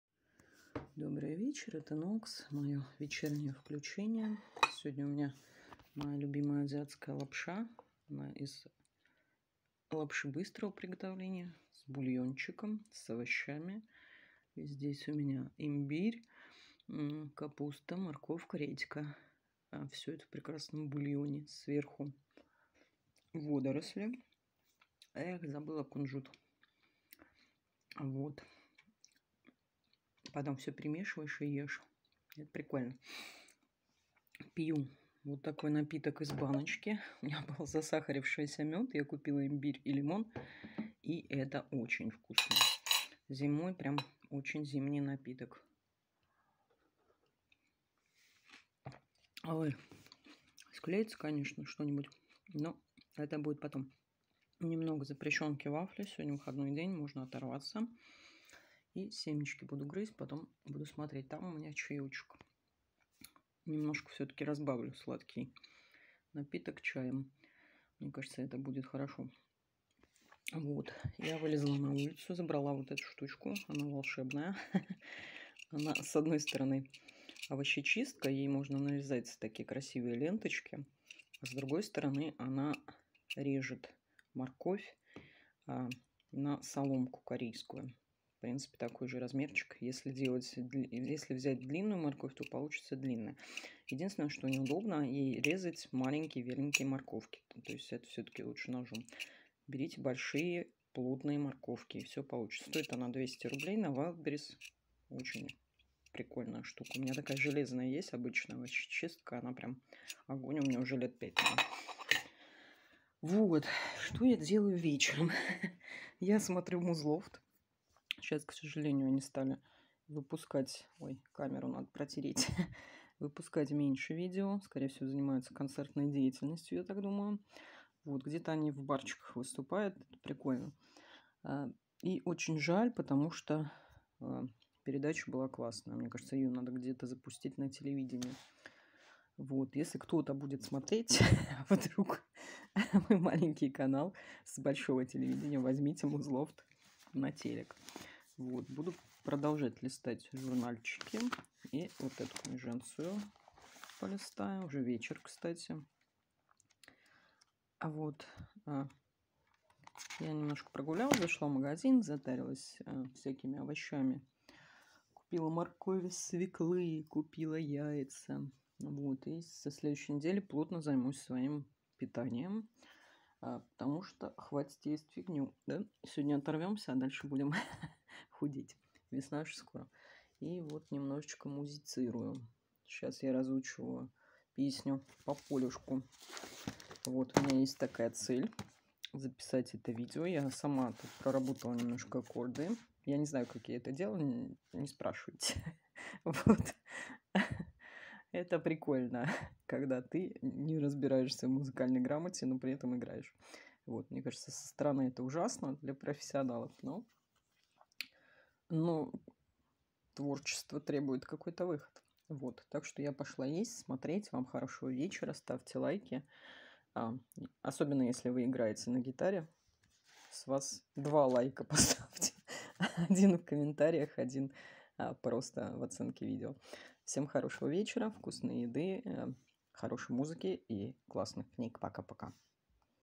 УВД: лапша по-азиатски, зимний чай, кухонные гаджеты и музицирование)
Помощь каналу (на книжки, оборудование, новую гитарку, культурмультурное развитие и макарошки))))

Телеграмм канал "Что делать"
Бусти: noxneoin
Росграмм: @noxneoin